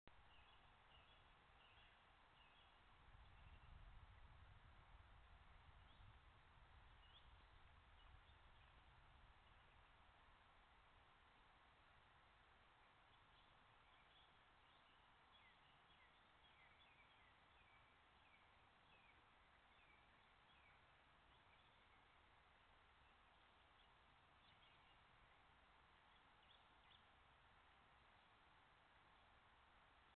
Frischlinge in der Morgendämmerung 5
Frischlinge kommen in der Morgendämmerung an die Kirrung